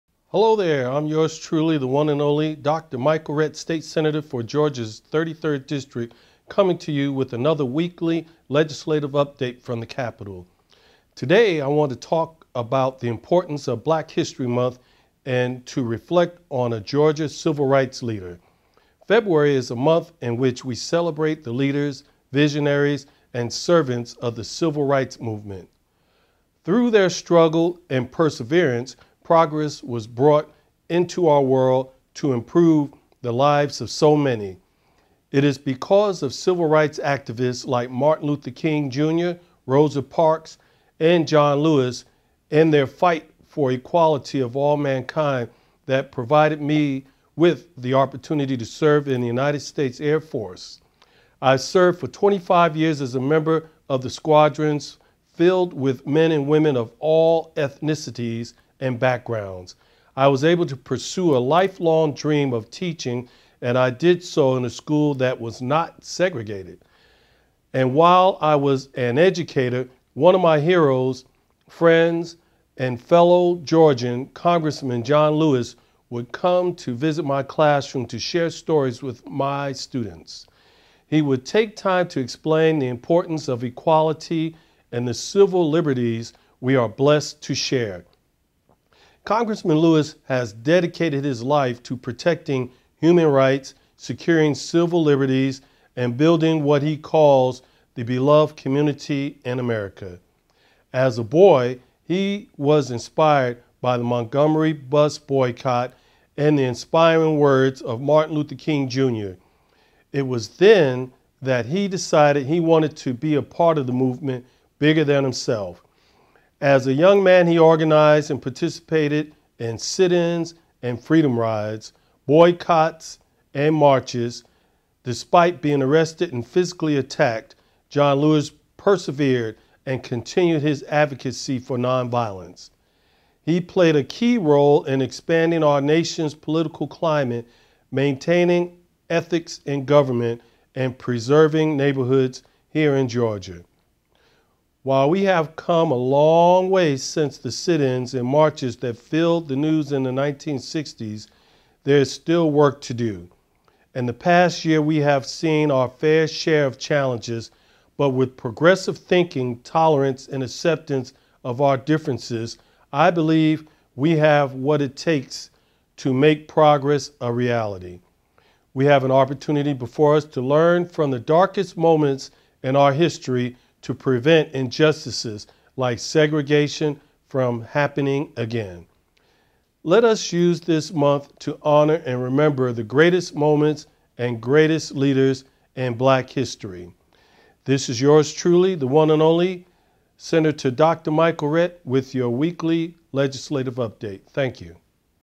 Sen. Rhett: Black History Month
Sen. Michael Rhett (D - Marietta) talks about the importance of Black History Month and his appreciation for Congressman John Lewis.